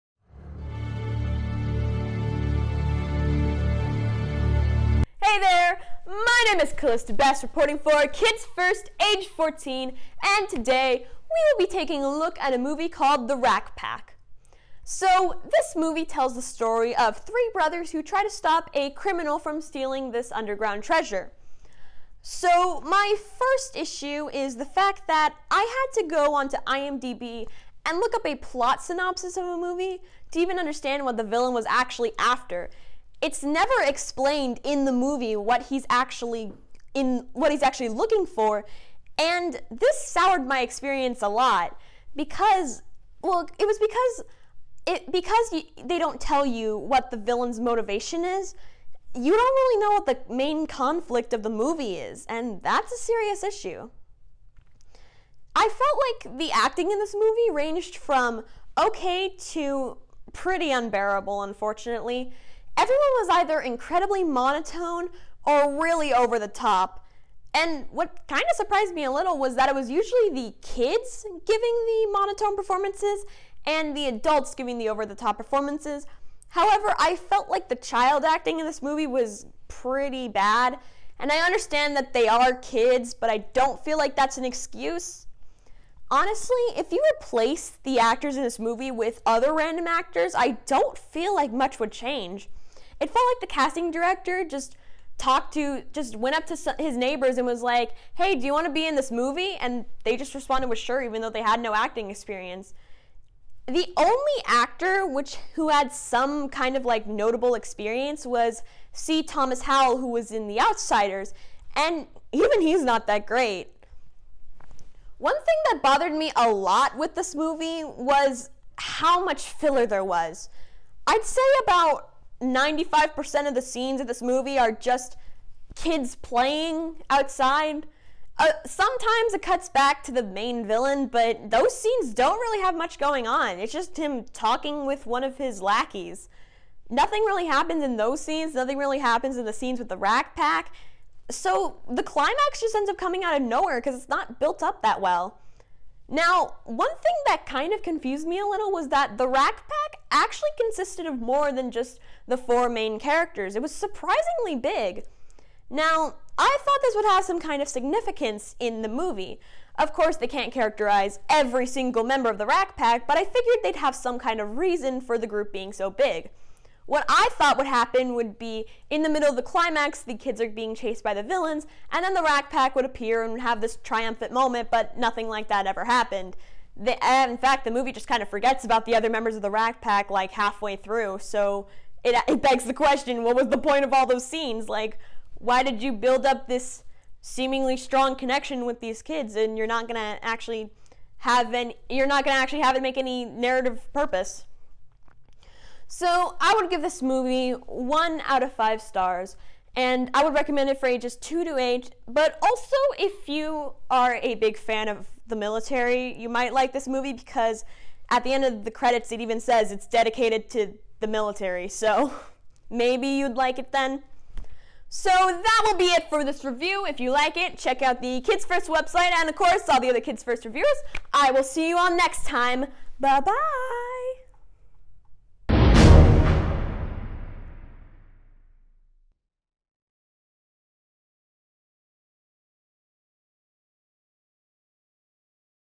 The Rack Pack by Calista B
Review of The Rack Pack by Calista B, Directed by Thomas J. Churchill; Writers: Darrell Rackley (book), Darrell Rackley (screenplay) and more. Starring C. Thomas Howell, Nico Ford, Hunter Lee Manning and more.